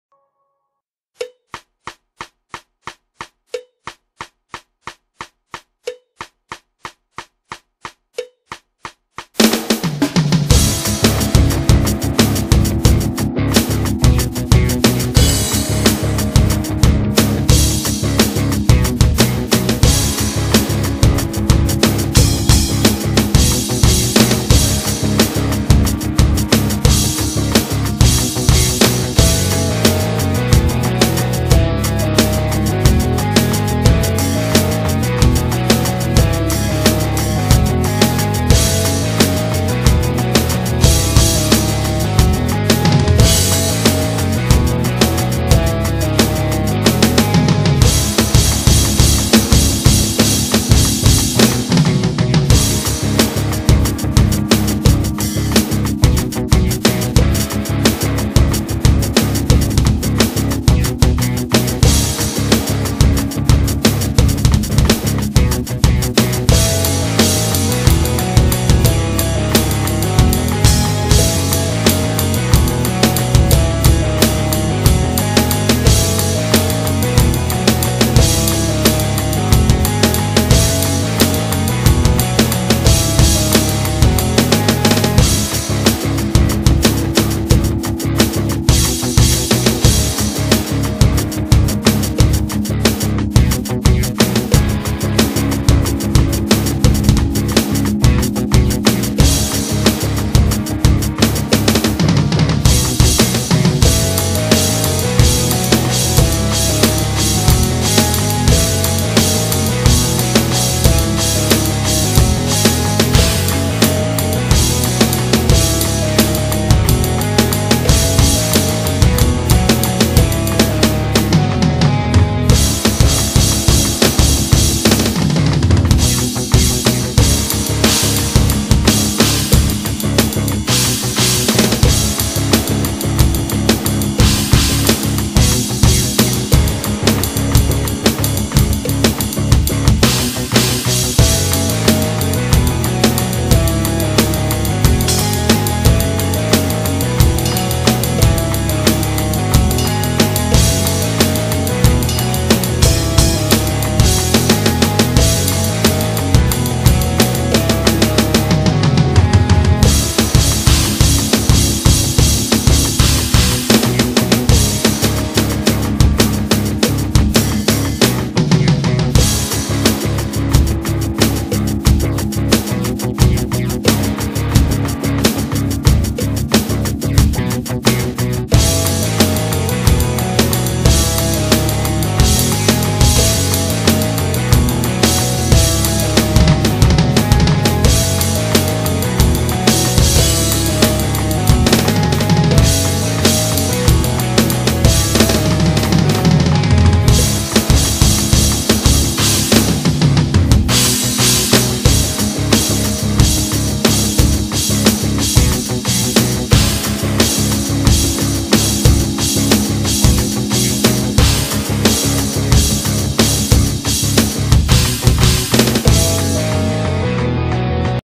DRUM COVER 7/8 RYTHM - VIRTUAL DRUMMING
PLAYING 7/8 RYTHM USING VIRTUALDRUMMING